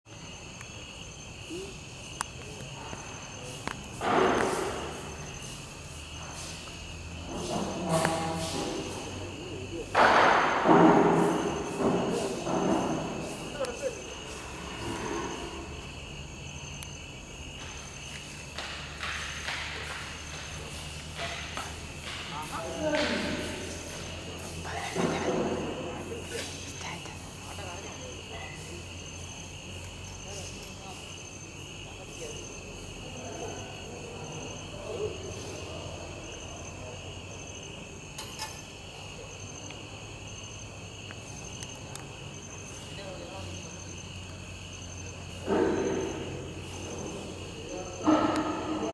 Watch ‘Super Blue Blood Moon’ Eclipse Crosses the Sky - Live From Kerala, India
Watch ‘Super Blue Blood Moon’ Eclipse Crosses the Sky - Live